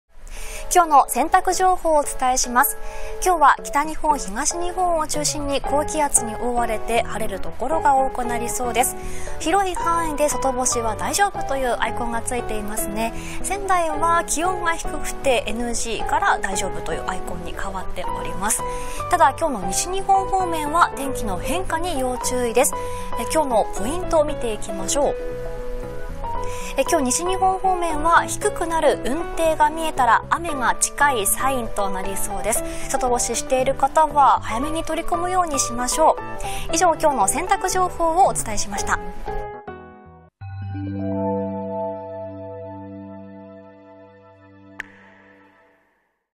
7日(日)洗濯天気 北日本から東日本は晴れて穏やか
北日本から東日本は高気圧に覆われて、晴れる所が多くなります。昨日6日(土)に比べると風は収まり、穏やかな天気になるため、洗濯の外干しもできそうです。一方西日本は空の変化を確認しながらの外干しとなりそうです。